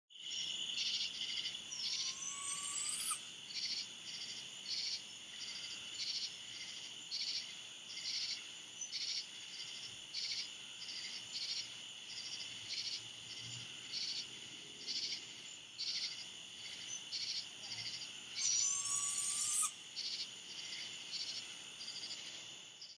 Juvenile Barred Owl
A juvenile barred owl calls at night near Tallahassee, Florida.